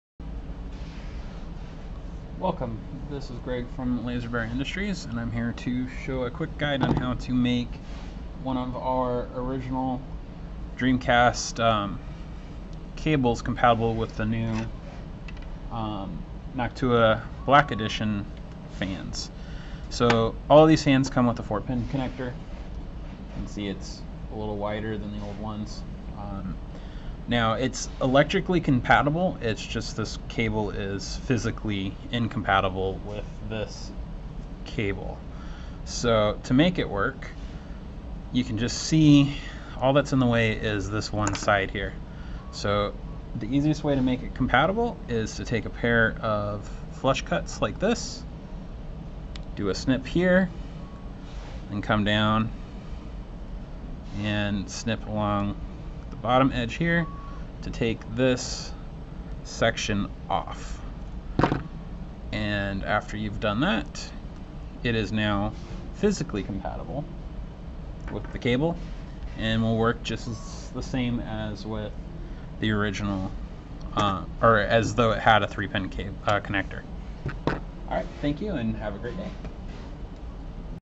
Modifying a the Dreamcast 3pin fan adapter cable to fit a 4 pin fan
This video shows hot to make the older fan cables compatible with the newer 4 pin fans offered by Laser Bear Industries